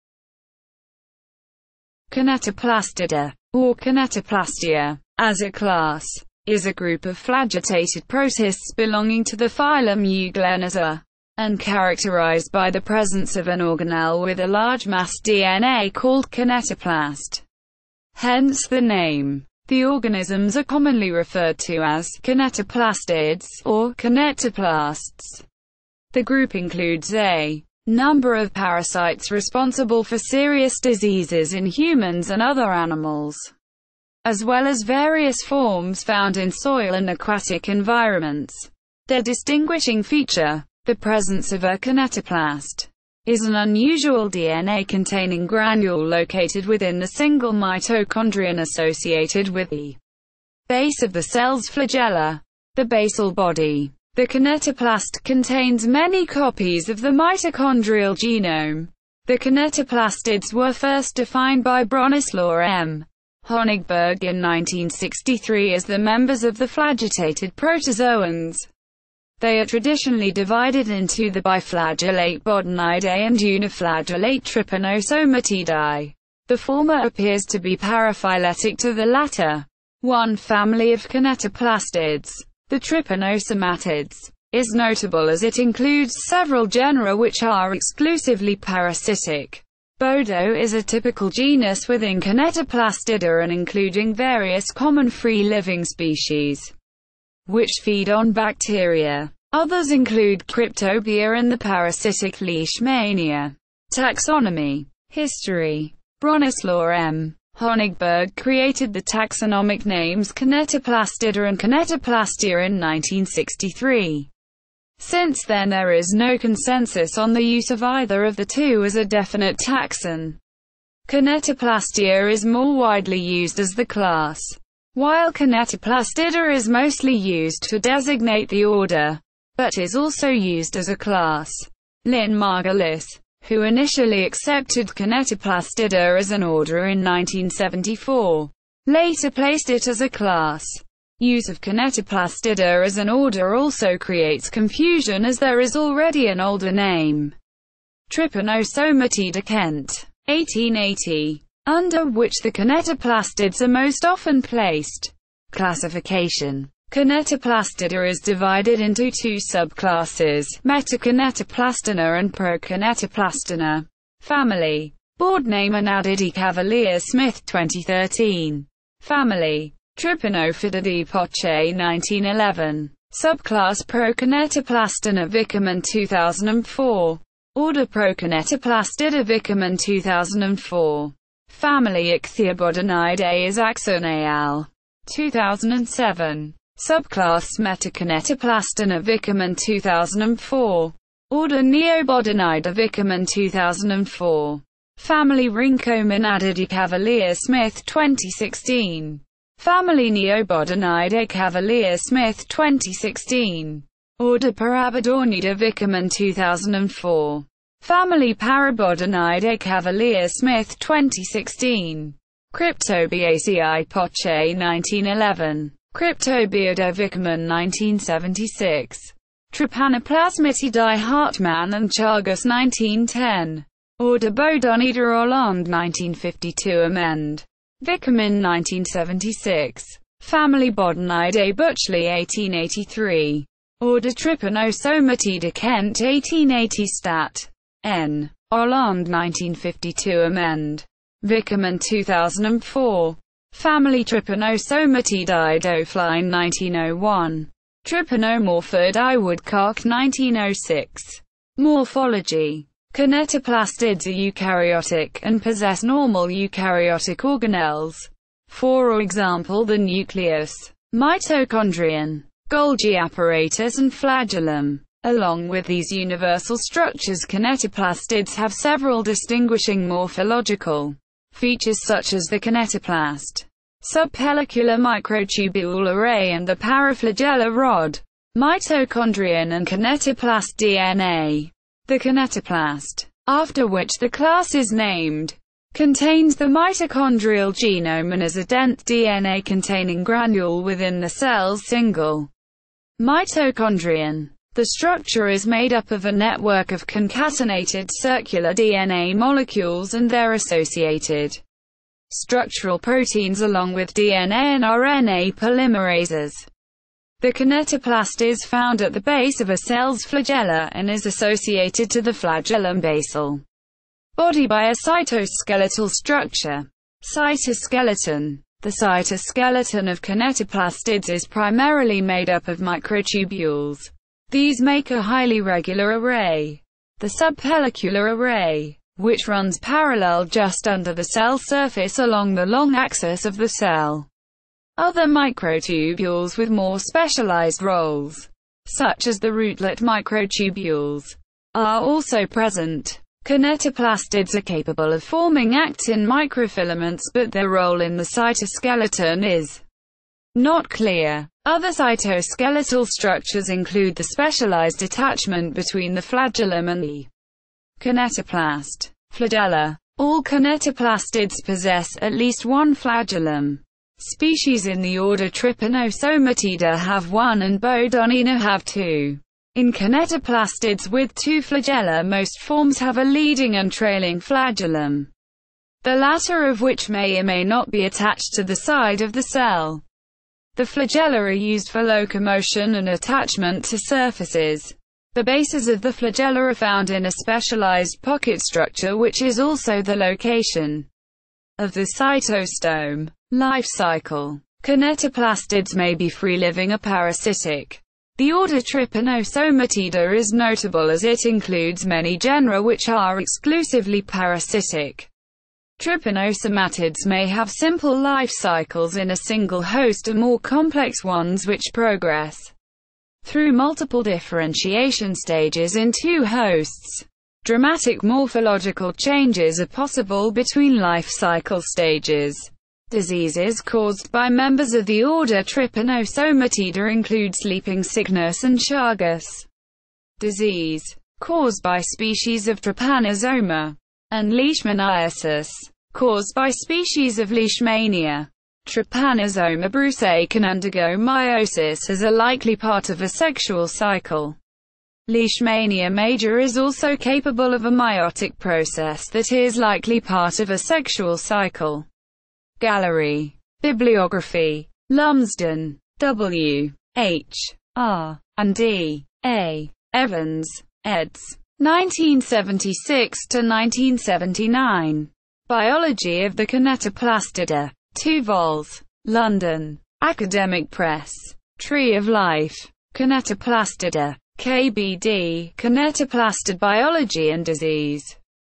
[Wikipedia] Kinetoplastida
Kinetoplastida (or Kinetoplastea, as a class) is a group of flagellated protists belonging to the phylum Euglenozoa, and characterised by the presence of an organelle with a large massed DNA called kinetoplast (hence the name). The organisms are commonly referred to as "kinetoplastids" or "kinetoplasts" The group includes a number of parasites responsible for serious diseases in humans and other animals, as well as various forms found in soil and aquatic environments. Their distinguishing feature, the presence of a kinetoplast, is an unusual DNA-containing granule located within the single mitochondrion associated with the base of the cell's flagella (the basal body). The kinetoplast contains many copies of the mitochondrial genome.
The kinetoplastids were first defined by Bronislaw M. Honigberg in 1963 as the members of the flagellated protozoans. They are traditionally divided into the biflagellate Bodonidae and uniflagellate Trypanosomatidae; the former appears to be paraphyletic to the latter. One family of kinetoplastids, the trypanosomatids, is notable as it includes several genera which are exclusively parasitic. Bodo is a typical genus within kinetoplastida and including various common free-living species which feed on bacteria. Others include Cryptobia and the parasitic Leishmania.